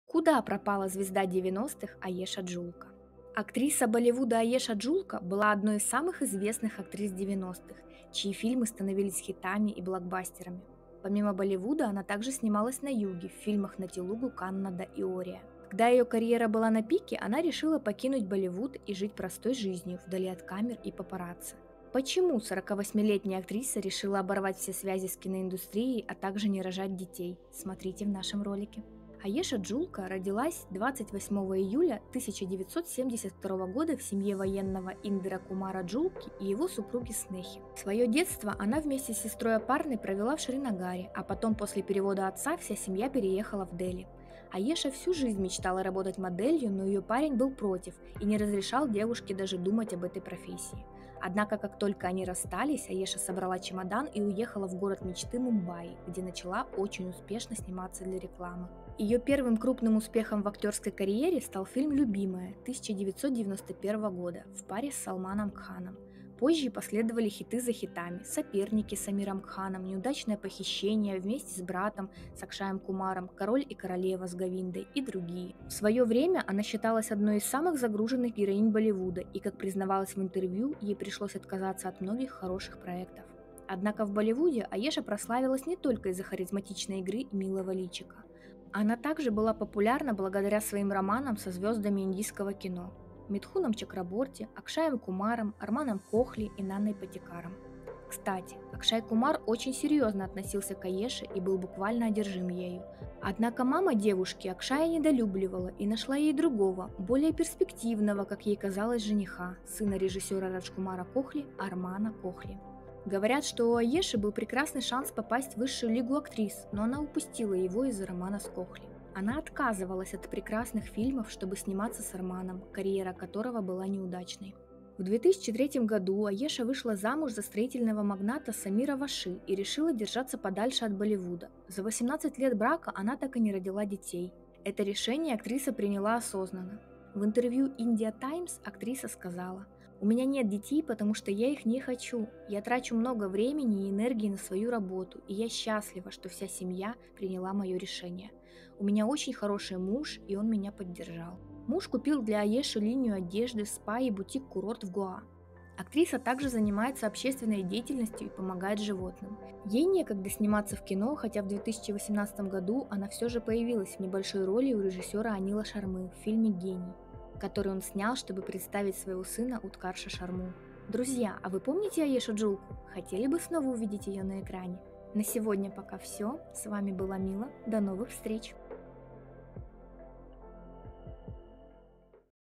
Куда пропала звезда 90-х Аеша Джулка?
Актриса Болливуда Аеша Джулка была одной из самых известных актрис 90-х, чьи фильмы становились хитами и блокбастерами. Помимо Болливуда она также снималась на юге. Когда ее карьера была на пике, она решила покинуть Болливуд и жить простой жизнью вдали от камер и папарацци. Почему 48-летняя актриса решила оборвать все связи с киноиндустрией, а также не рожать детей — смотрите в нашем видео.